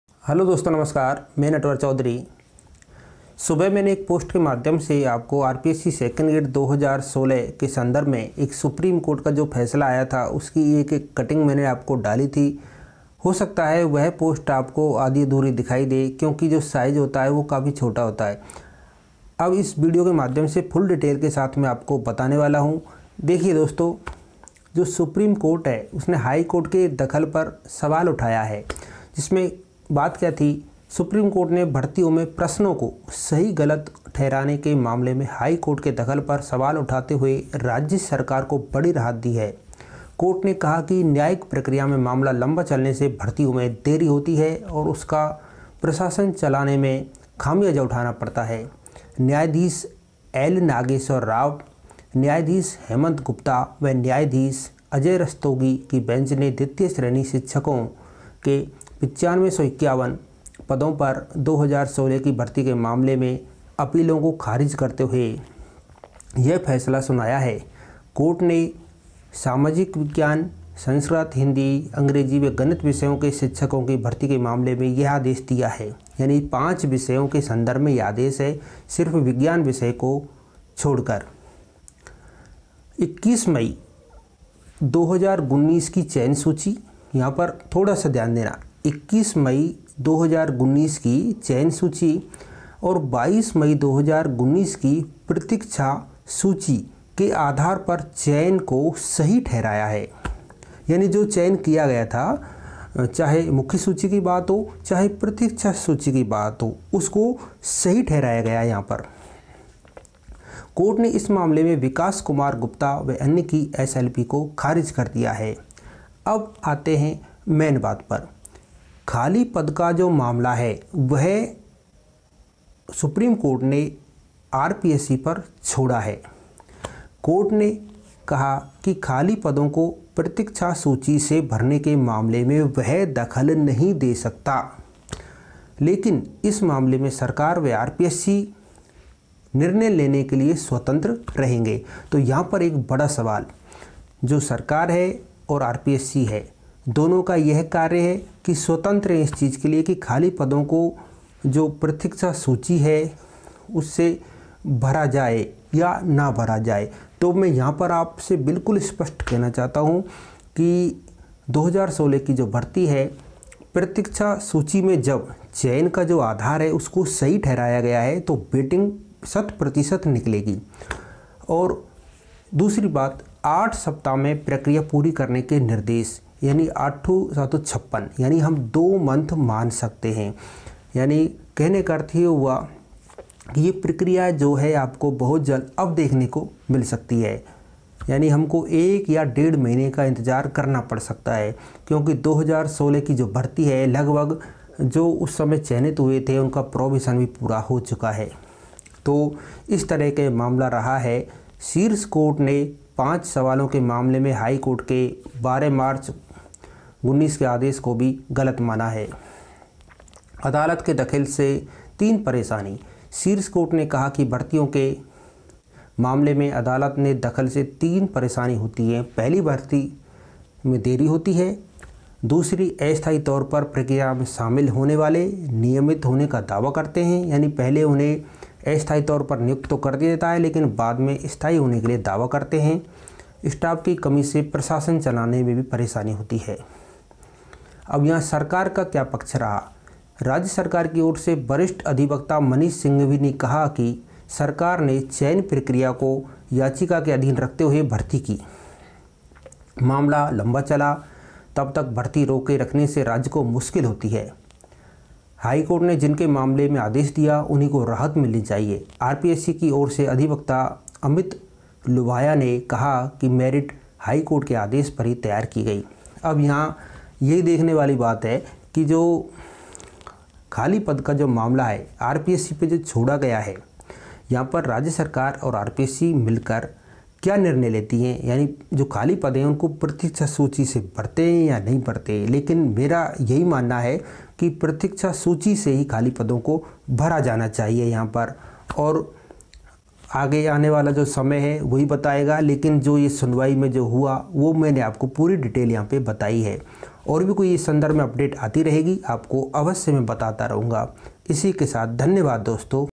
Rpsc 2nd grade 2016 | सुप्रीम कोर्ट सुनवाई अपडेट | प्रतीक्षा सूची के संदर्भ में big update
Regard
Natver chaudhary